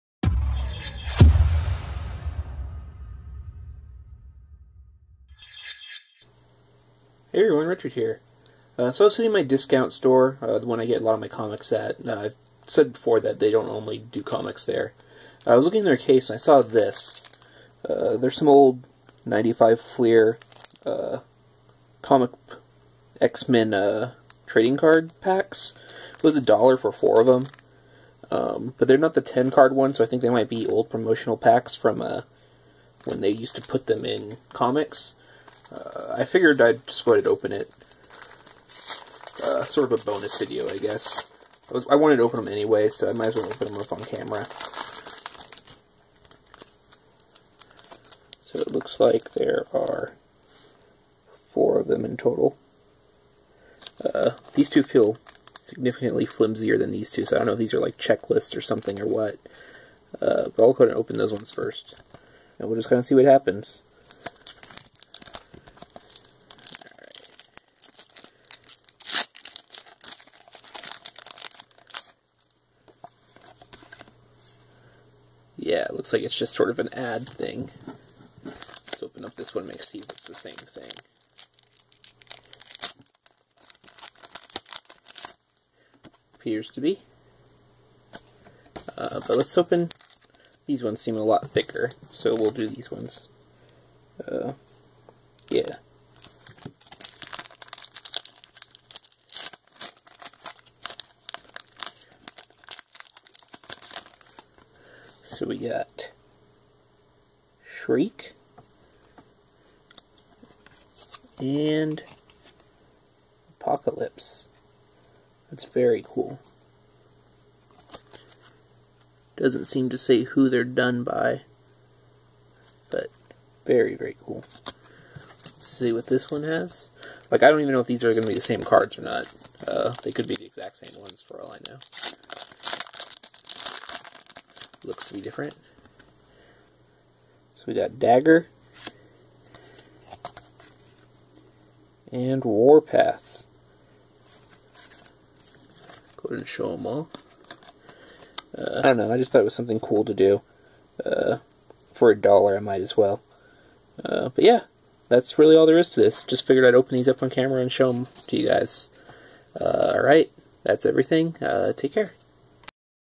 Opening Up 90s X-Men Trading Cards!
Figured for a dollar I might as well see what these were.